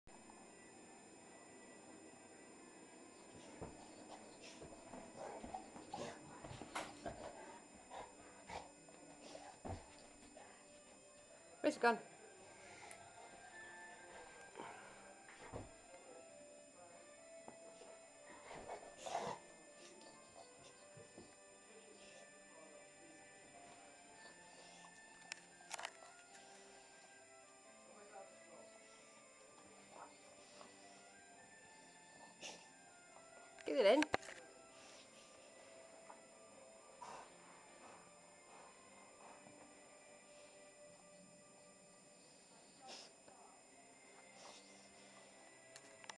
Dogue de Bordeaux with her biscuit
My 2 year Dogue de Bordeaux does this at least once a day and always ends her loony antics with a smile